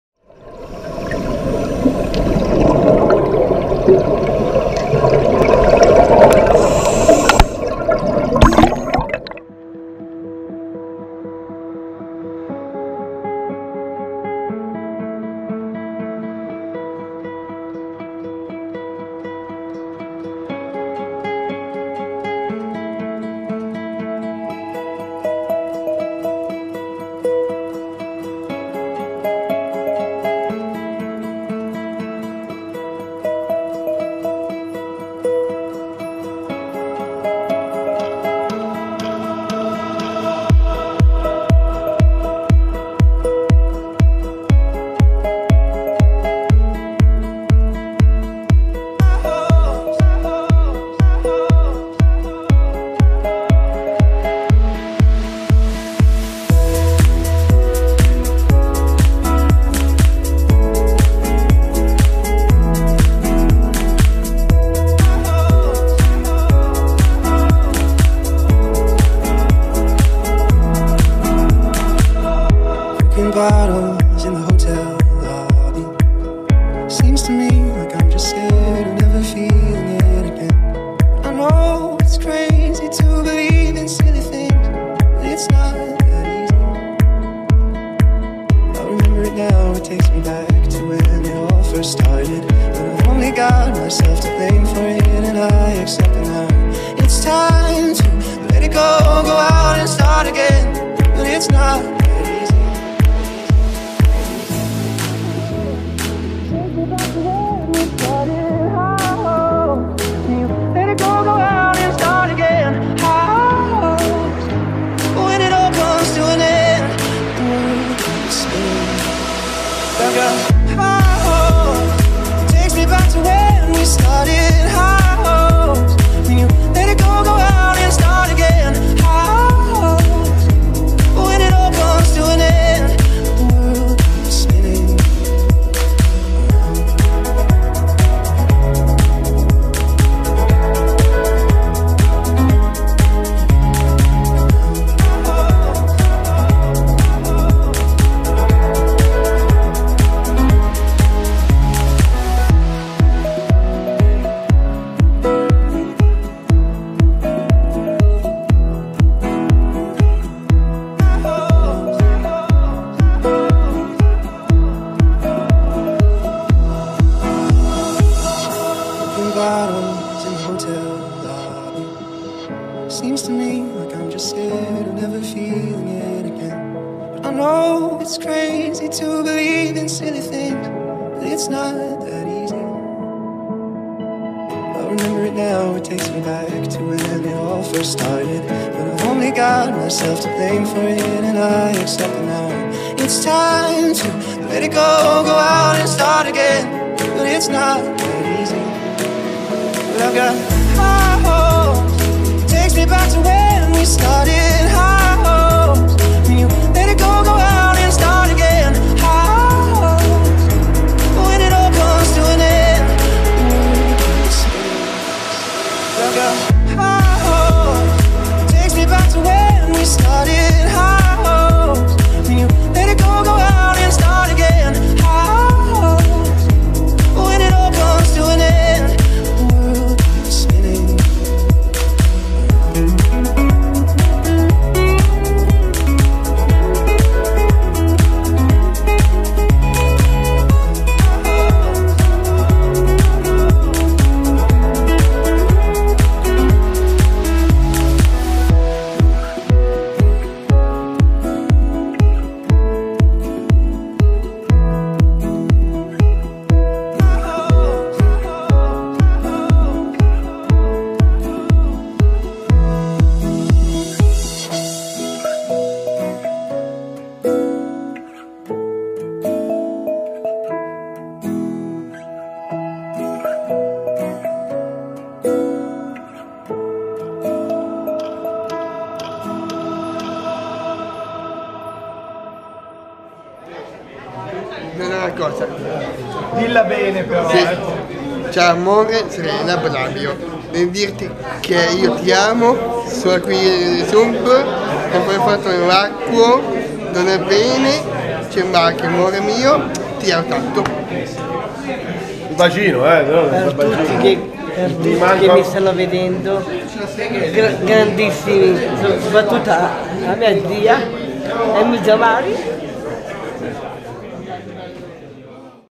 Y-40 - 13 subacquei con disabilità alla piscina più profonda del mondo | DDI Italy
Il bellissimo racconto dell'immersione di 13 ragazzi con disabilità alla Y-40 The Deep Joy, la piscina più profonda del mondo a Montegrotto Terme (PD).

Un giornata indimenticabile per 13 neo brevettati DDI iniziata all'alba di domenica 8 ottobre 2017 con il viaggio in pullman alla volta di Montegrotto Terme (PD), per andare ad esplorare la piscina dei record, la Y-40!
Tanta emozione anche per i loro istruttori che li hanno accompagnati nel percorso di scoperta del nostro meraviglioso sport..
Una grande festa per DDI e per la subacqueapertutti che oltre a coronare il sogno dei nostri neosubacquei è stato un momento d’incontro e condivisione per i tanti professionisti DDI prima dell’inizio del nuovo ‘anno accademico’.
E che dire di parenti e amici che hanno fatto il tifo ai ragazzi e hanno avuto la possibilità di vederli sott’acqua dal tunnel trasparente che attraversa tutta la vasca?!
A condividere quest’esperienza è stata presente con noi DAN che ha coinvolto i ragazzi in una lezione sulla sicurezza in immersione.